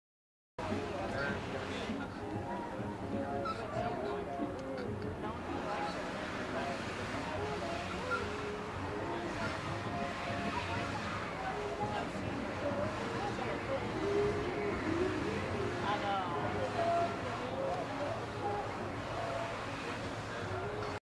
Sea lions sleeping on a boat
I saw a family of sea lions happily sleeping on a docked boat at the Santa Monica Pier and decided to record a short clip of them.